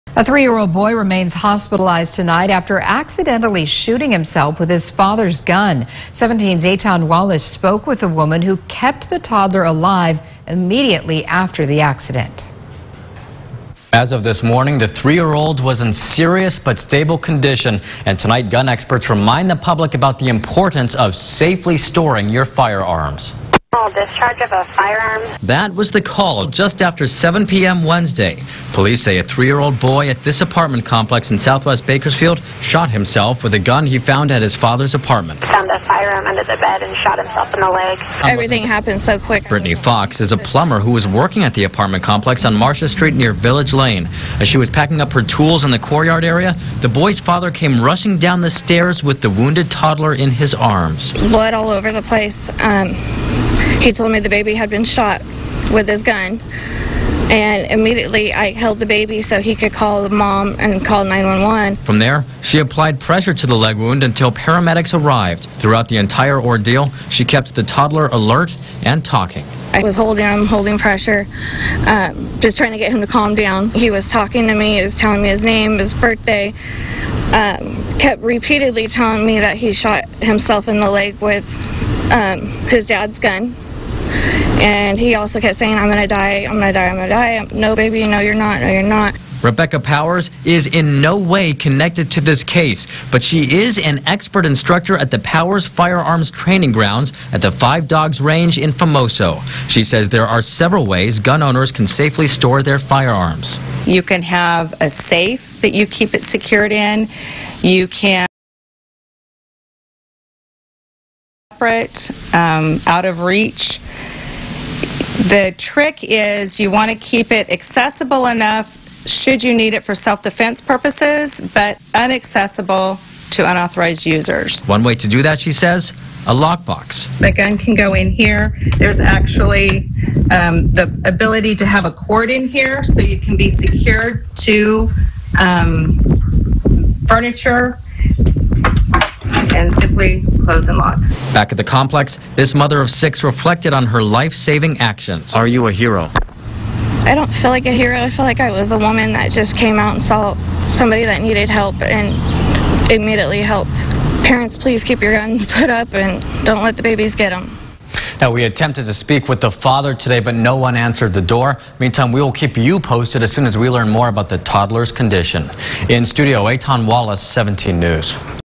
3-year-old managed to find gun, shoot himself, police say
3-year-old managed to find gun, shoot himself, police say